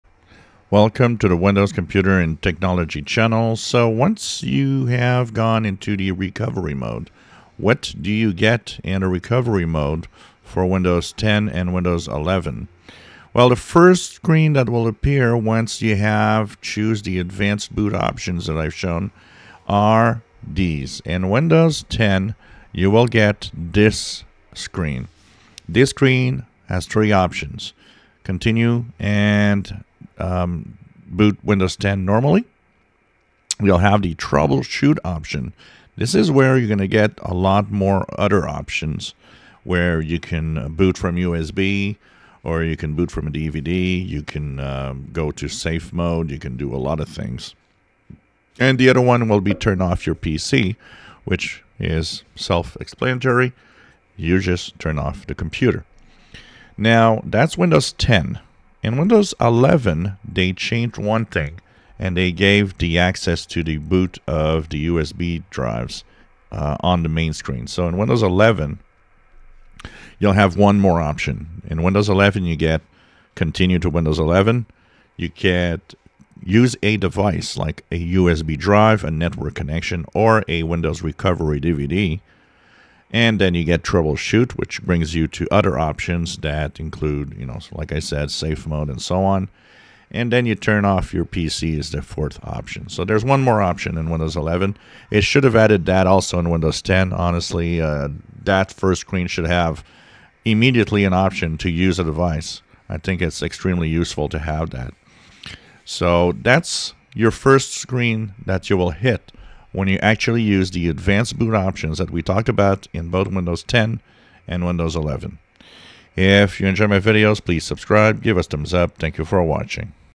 Windows 10 11 First screen Advanced boot up options recovery
The first screen when you boot into the Advanced options in Windows 10 and 11